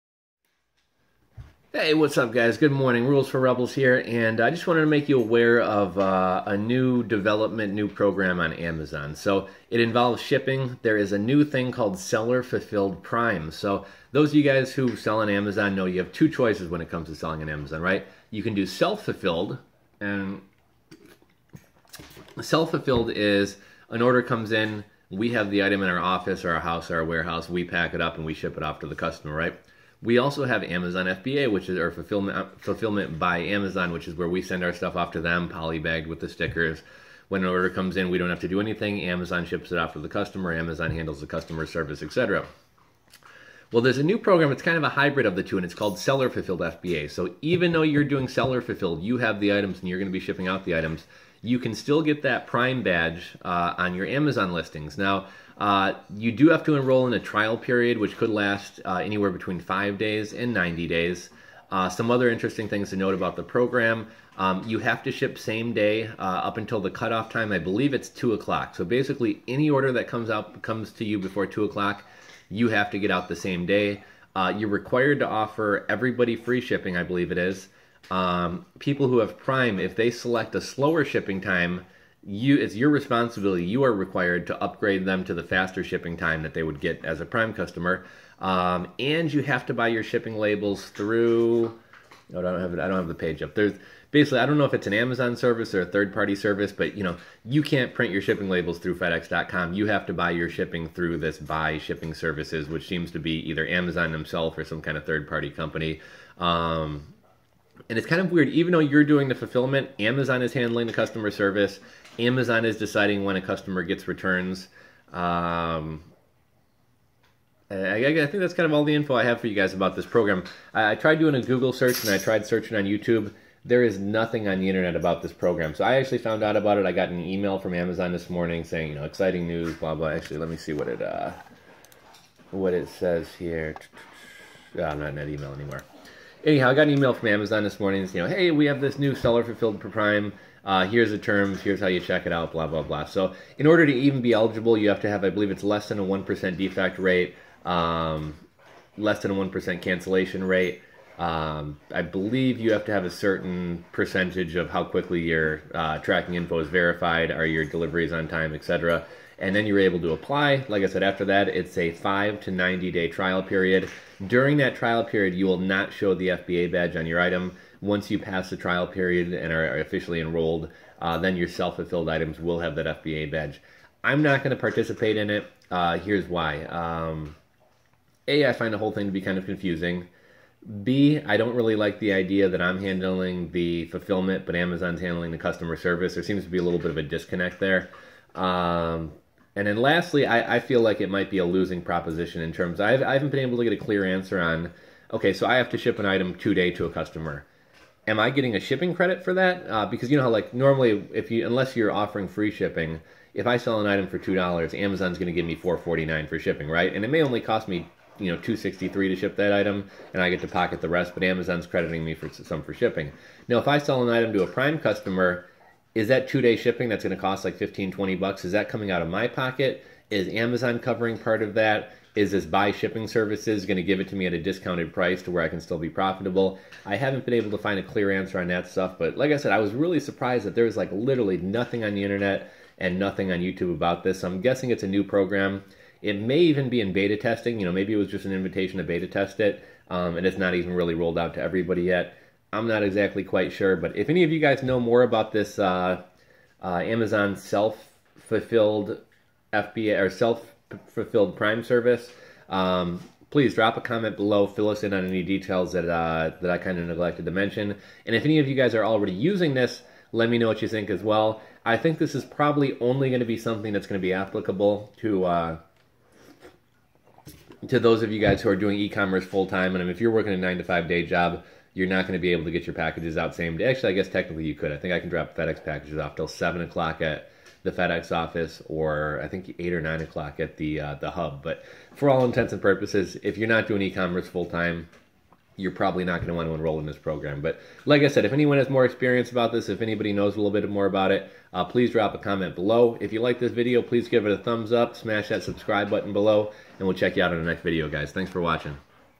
New Amazon Self Fulfilled FBA - How Your Self Fulfilled Items Can Be "Prime" Items
Hey guys, I'm still a bit hazy on the details of this program, however I got an e-mail from Amazon the other day announcing a new feature called Self Fulfilled Prime.

For many of you this may not be a viable option as you need to offer free 2 day shipping and if you're not a full-time ecommerce person it may be difficult to do while also having a day job. That said just wanted to make you guys aware of it.

Essentially your items can have the "Prime" banner and prime shipping time on them even if they aren't in Amazons  warehouse and you are self fulfilling.

There is a lot of red tape to this program understandably since AMazon customers have come to expect a certain level of service and expediency with Prime.

You need to agree to ship same day til cutoff time. You also need to buy your shipping through a specific Amazon program or vendor.

You also must go through a 5 to 90 day trial period where you are participating in the program but don't have the banner, essentially to prove you can meet their standards and expectations.

Personally I don't think I'm going to be part of this program. While it is a great perk being able to appeal to Prime customers without incurring all the FBA fees, I think you can ship cheaper yourself than with FBA fees, however I see too many headaches arising being part of this program so personally I'm probably not going to participate, at least not until I hear some others people's experiences.

I apologize the video lacks a lot of details, I just learned of this program, there's really no articles or videos out there about it yet but just wanted to make you guys aware.